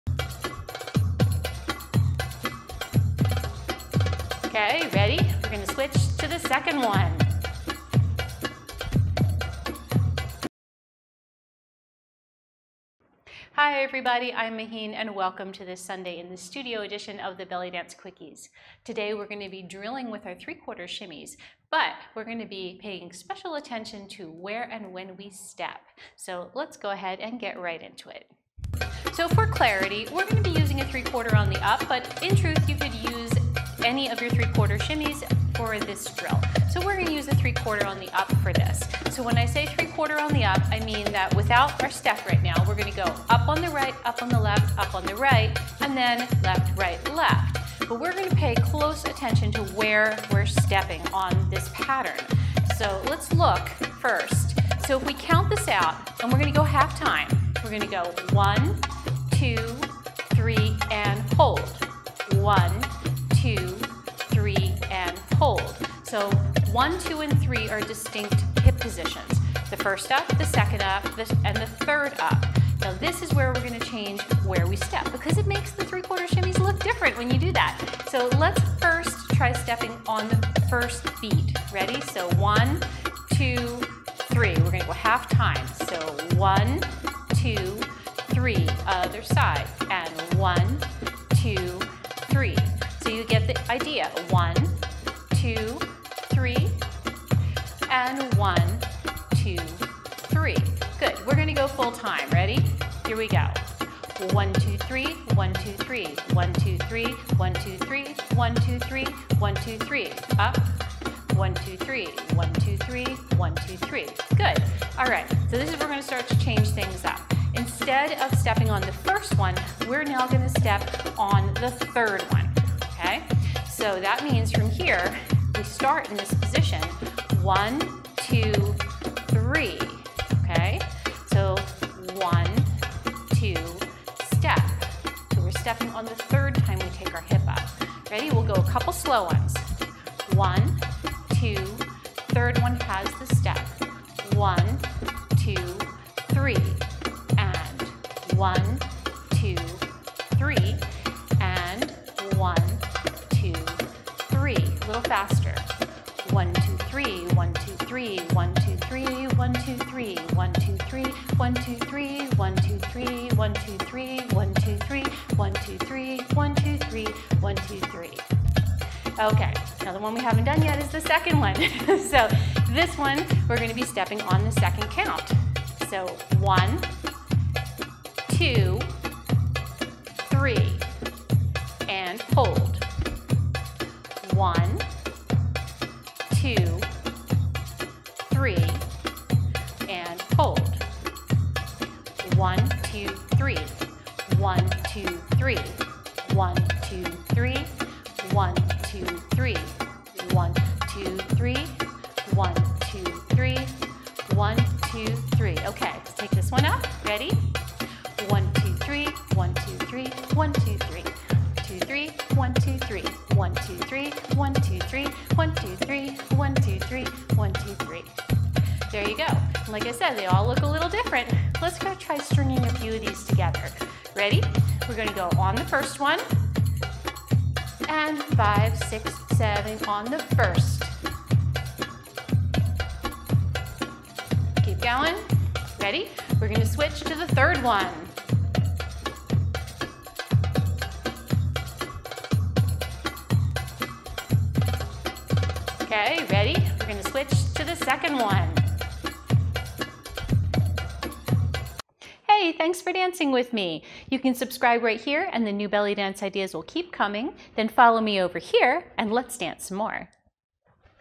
⭐Belly Dance: 3/4 Hip Shimmy Drill Challenge! ⭐
Belly dance shimmy drill for 3/4 walking hip shimmy. Intermediate bellydance practice drill.  Vary the look of your 3/4 shimmy by changing your stepping pattern. Try it with other walking 3/4 shimmies too!

There's lots more bellydance learning & inspiration waiting for you in Mahin's Bellydance Quickies email!
Belly dance tutorials, costume tips, culture, music and more!
Mahin